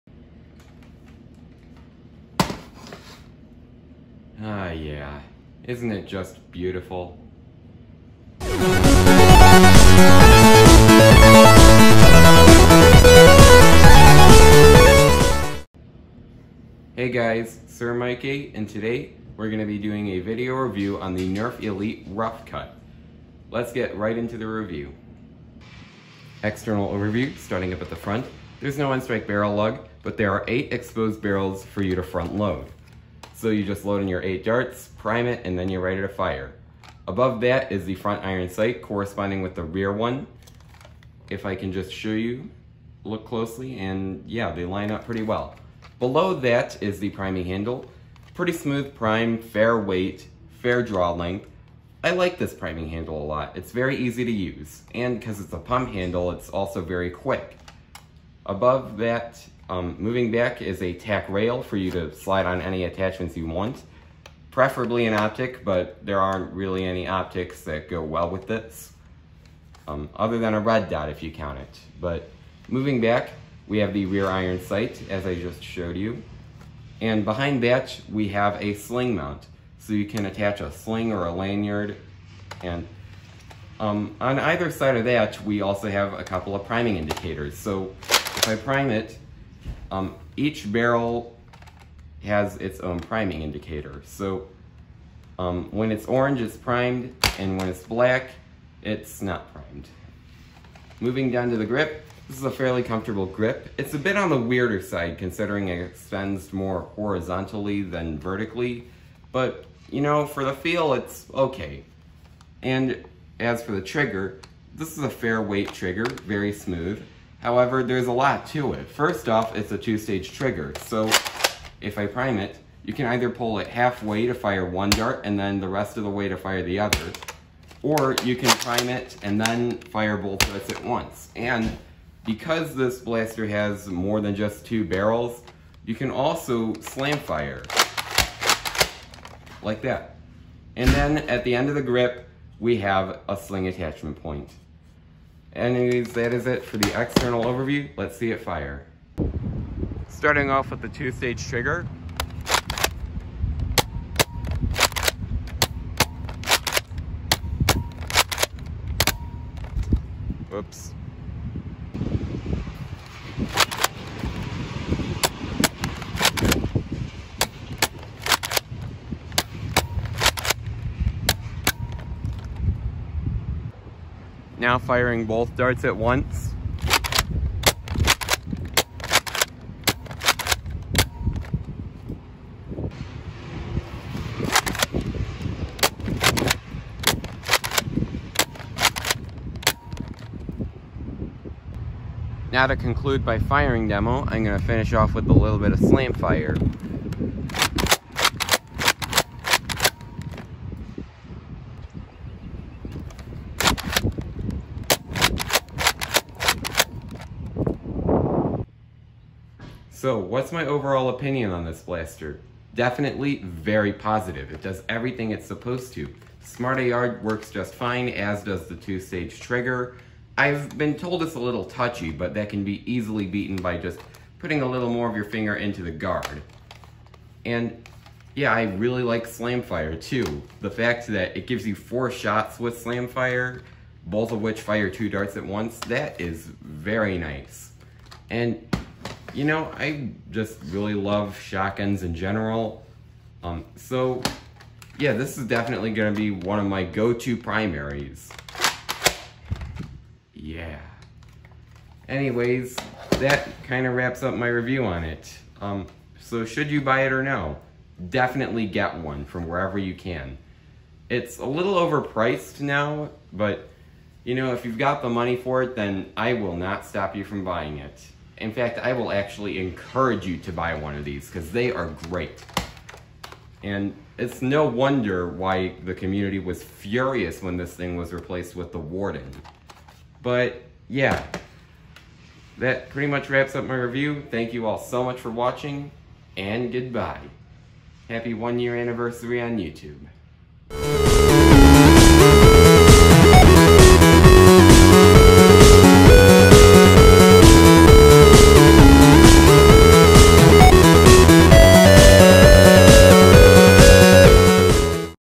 Nerf Rough Cut Review + Outdoor Firing Demo!
Hey guys! I'm back for my first anniversary, and I've got quite a good blaster to review! It's the most beloved of Nerf shotguns--the Rough Cut!

The Rough Cut is a shotgun-style blaster that put the Barrel Break to shame. But is it worth buying overall? Unsurprisingly, yes. Watch the video to find out why.

I won't be including a link to buy it, because I can't guarantee that it'll still be in stock for most sellers. Just type "Nerf Rough Cut" into the search bar, and you should find some eventually.

As always, you're a legend if you read the description!